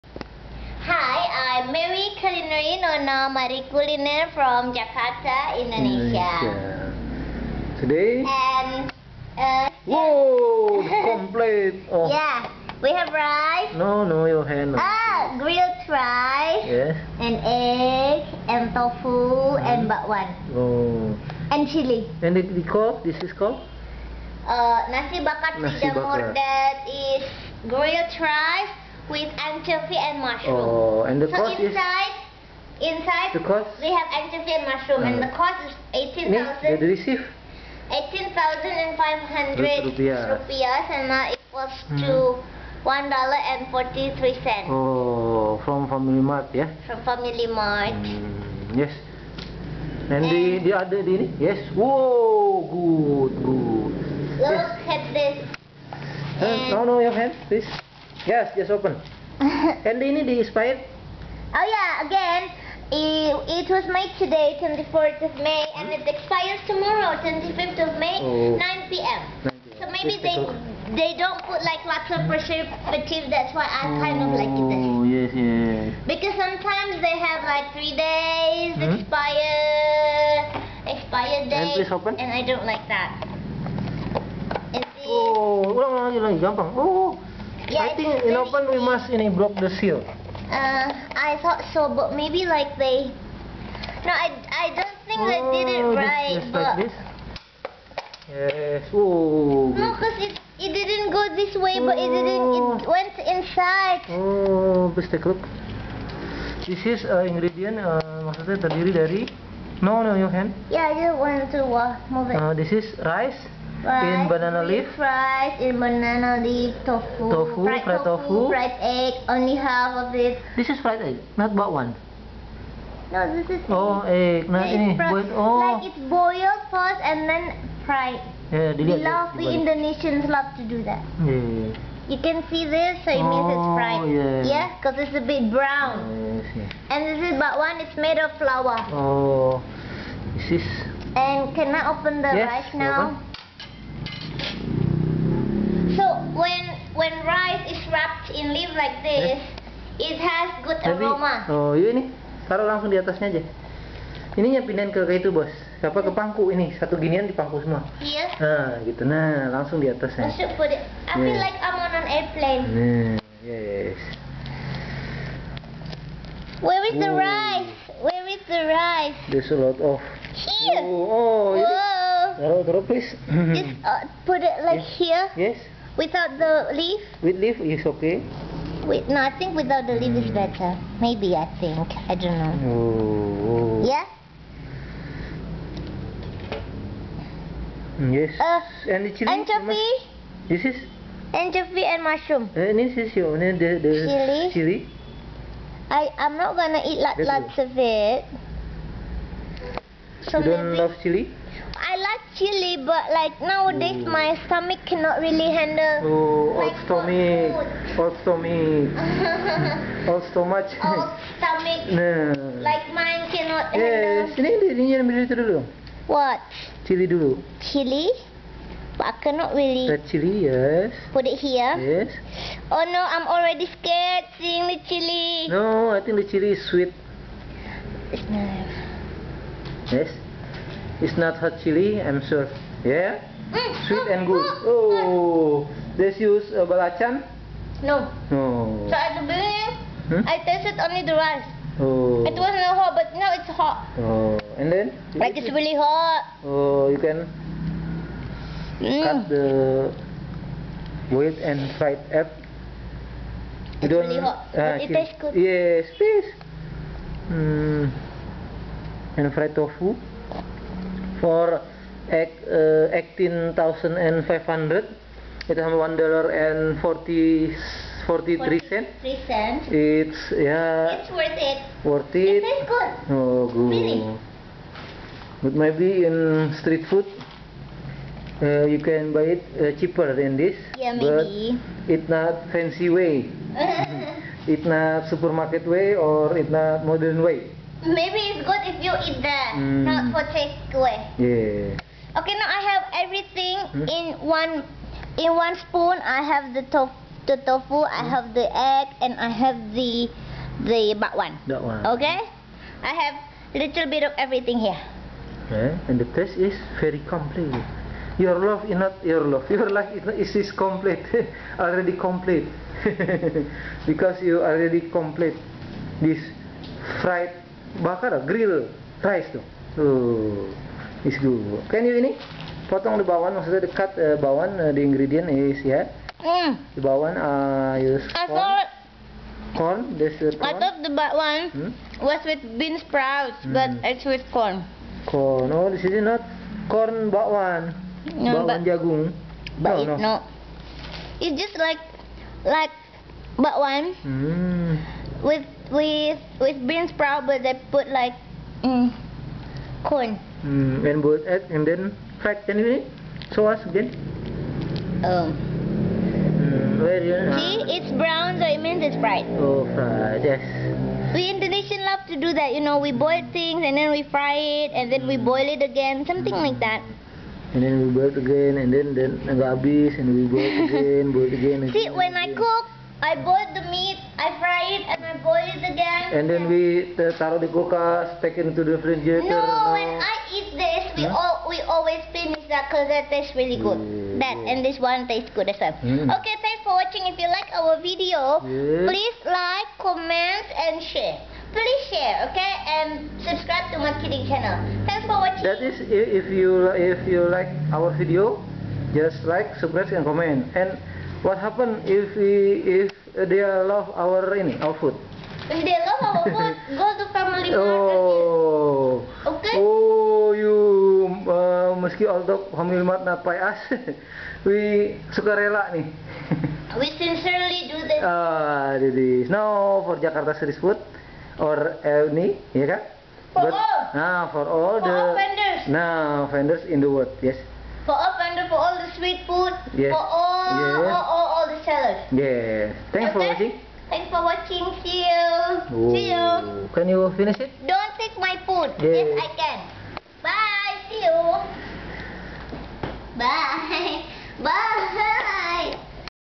Miss Mary Culinary (English Language) Food Review 71 Complete Grilled Rice FamilyMart 3228 3406
Nona Mari Kuliner Food Review Nasi Bakar FamilyMart Jakarta Indonesia Perumnas Klender Jakarta Timur.